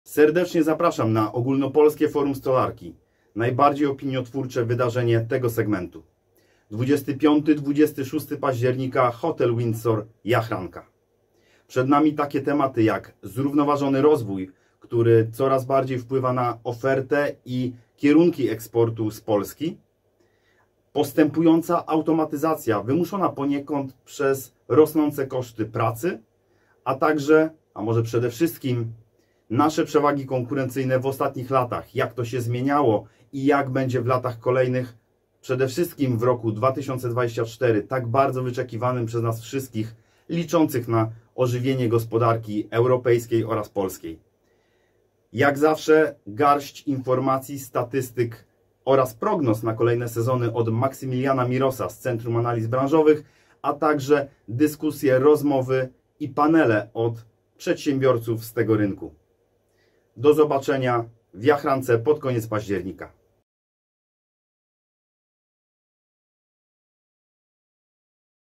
Karol Klos (Forum Branżowe) zaprasza na Ogólnopolskie Forum Stolarki 2023
❗️Serdecznie zapraszamy na tegoroczne Ogólnopolskie Forum Stolarki❗️

Karol Klos poprowadzi debatę o konkurencyjności polskiej stolarki i automatyzacji produkcji 🦾 w której głos zabierze m.in. Kryspin Baran - prezes zarządu firmy Aluron, partnera głównego tegorocznego spotkania 🤓